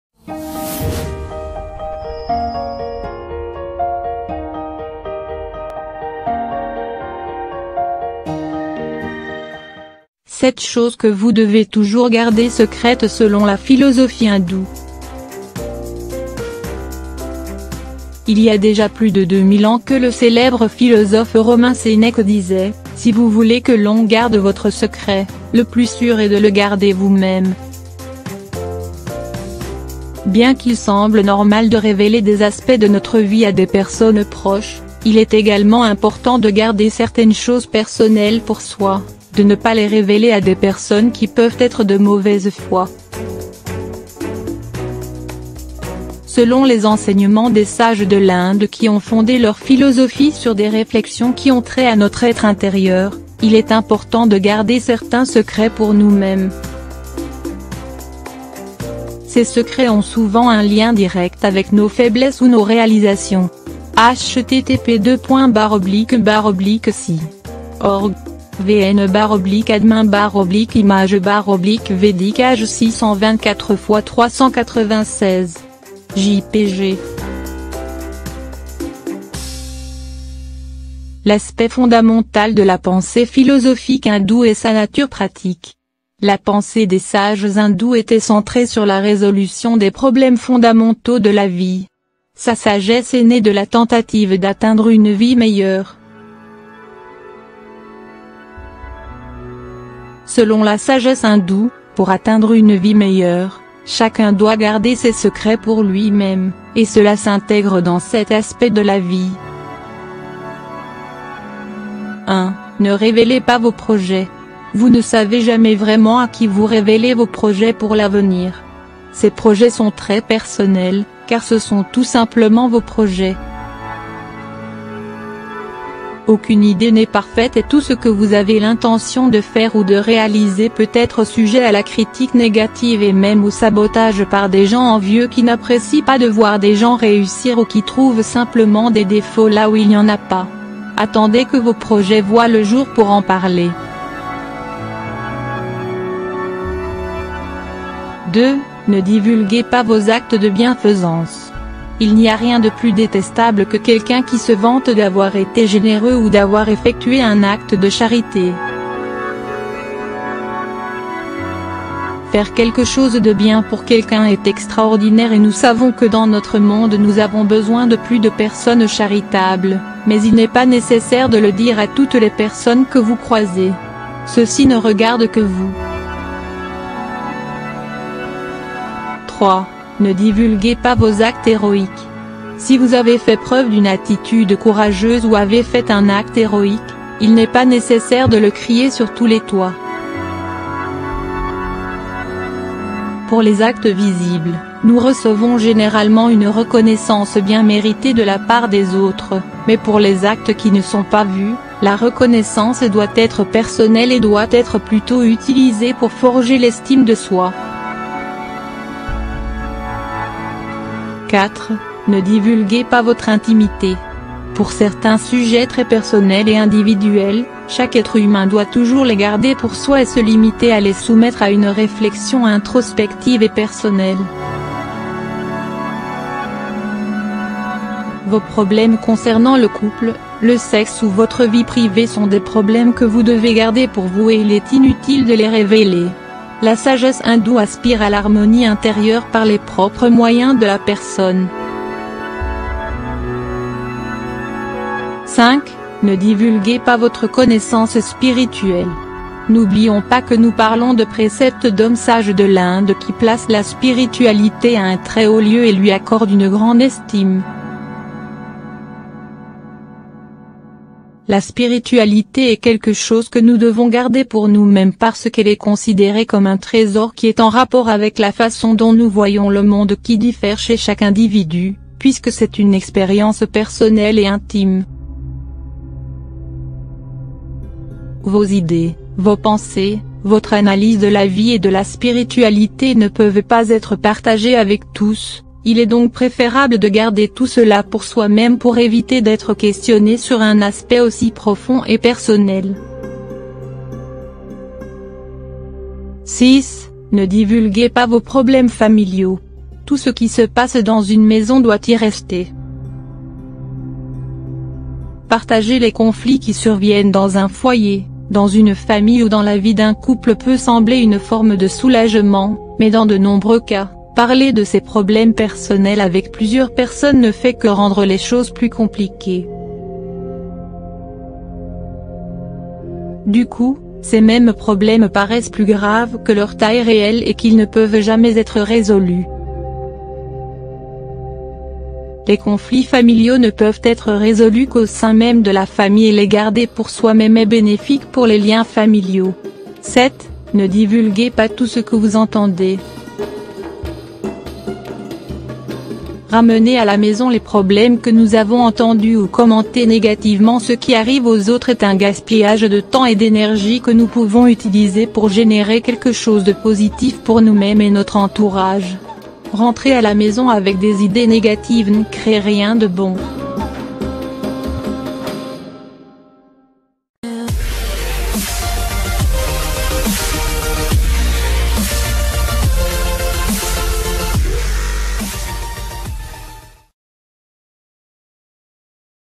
7 choses que vous devez toujours garder secrètes selon la philosophie hindoue
Il y a déjà plus de 2000 ans que le célèbre philosophe romain Sénèque disait : «Si vous voulez que l'on garde votre secret, le plus sûr est de le garder vous-même.». Bien qu’il semble normal de révéler des aspects de notre vie à des personnes proches, il est également important de garder certaines choses personnelles pour soi, de ne pas les révéler à des personnes qui peuvent être de mauvaise foi.

Selon les enseignements des sages de l’Inde qui ont fondé leur philosophie sur des réflexions qu